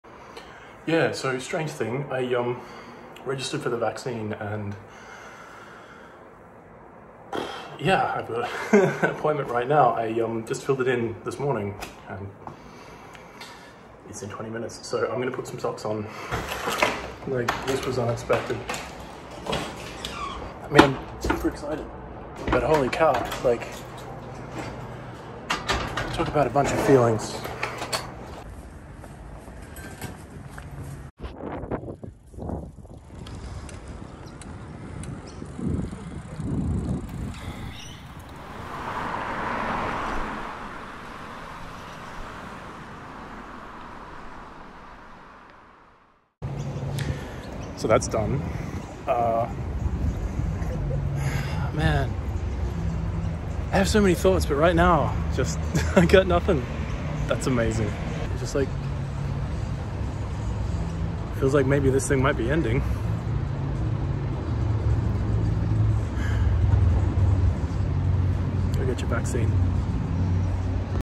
Get vaccinated or be square
The ABC's top headline this morning: “Doctors call for better COVID-19 vaccination promotion, warning Australians are 'sitting ducks' until they're vaccinated”.

I've been periodically checking the vaccine eligibility site, but today was the first time it would let me book. RBWH had doses.

So I accidentally booked one _right now_ and had to rush over. Feeling very grateful and lucky right about now.